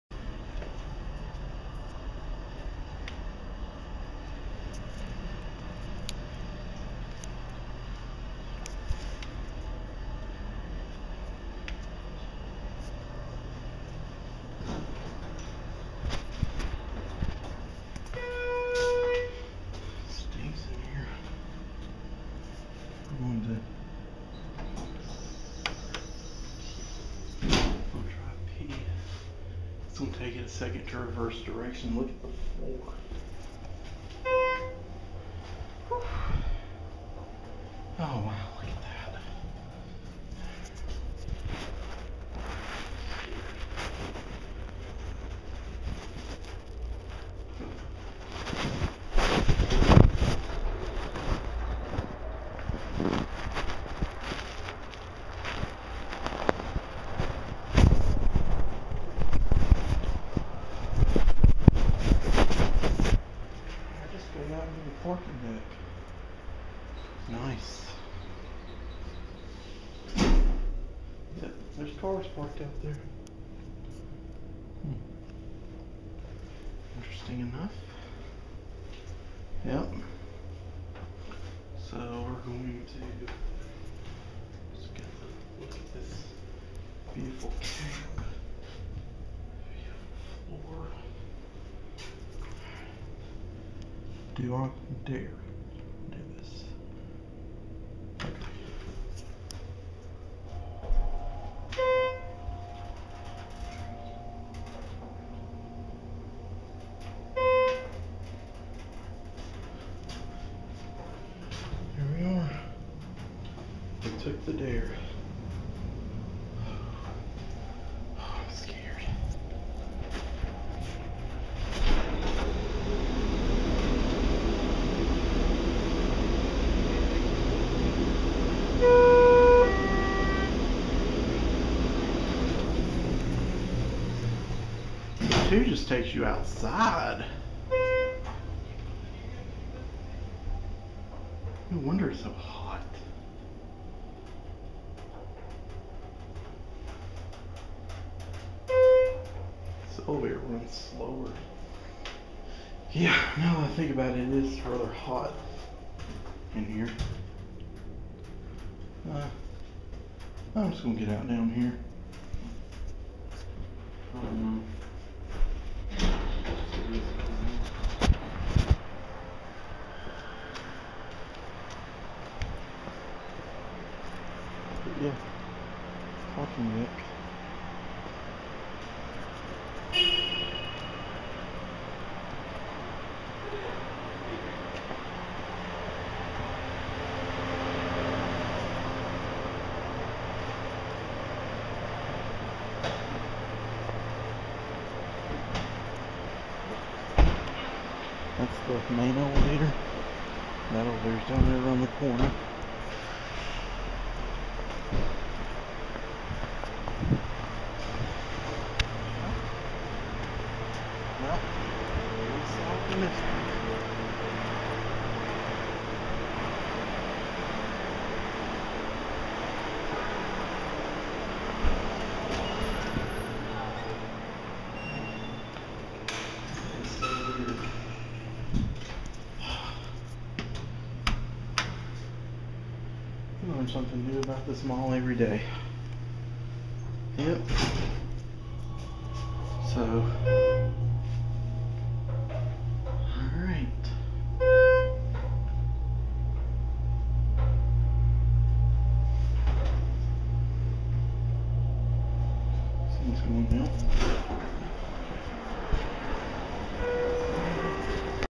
Schindler Hydraulic Service elevator at Colonial Brookwood Village.
This elevator stunk today. I now know why it is hot inside - the elevator opens up to the outside on 2 behind the mall.